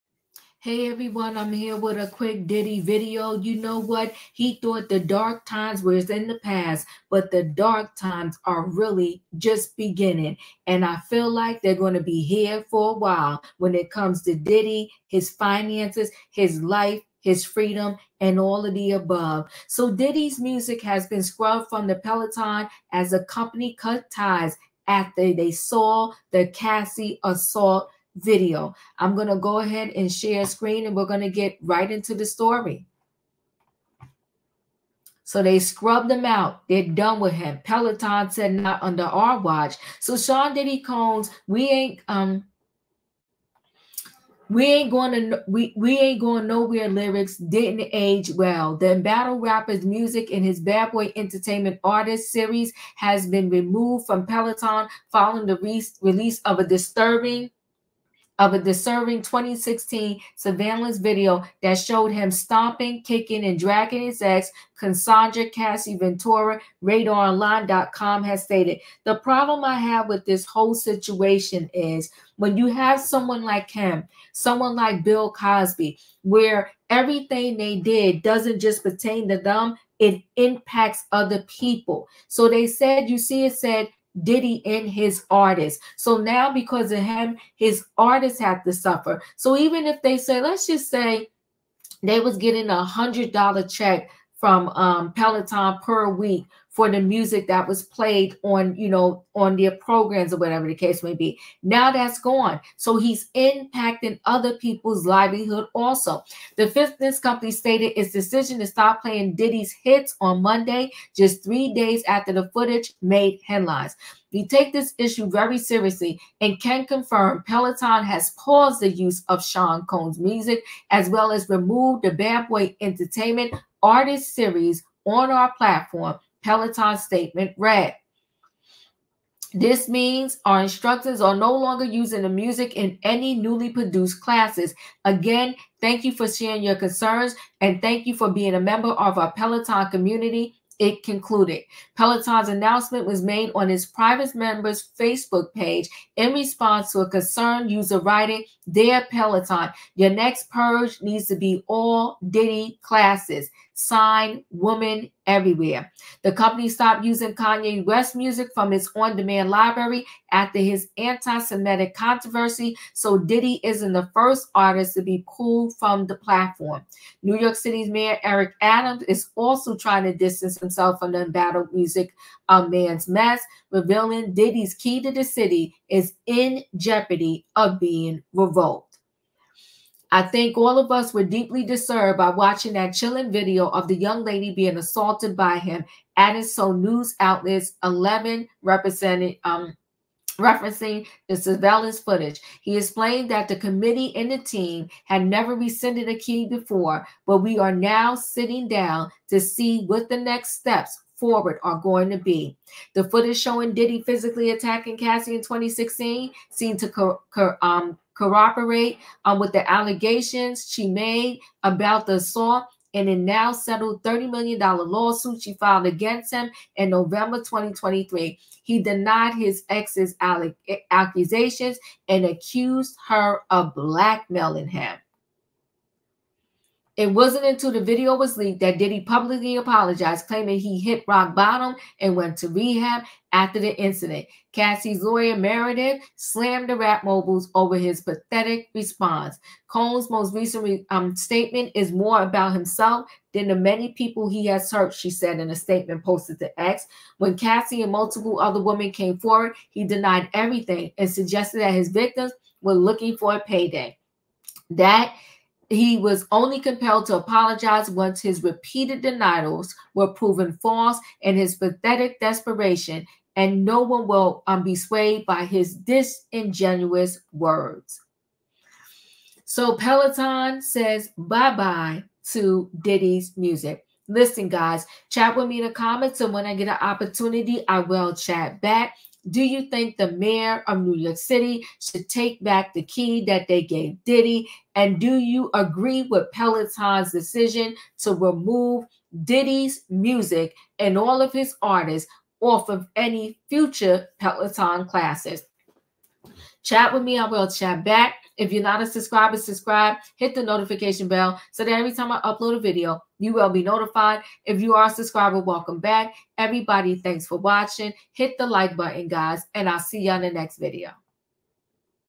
Peloton Drops Diddy's Music| No Tolerance for Violence| HIS ARTIST TAKE THE HIT ALSO
In a recent move, Peloton has decided to remove Diddy's music from their platform, citing a zero-tolerance policy towards violence. This decision follows their earlier action of removing Kanye West's music. While Peloton aims to uphold its values, this move unfortunately affects not just Diddy but also all the artists associated with him.

Join us as we delve into the implications of Peloton's decision, how it impacts the music industry, and the response from the community.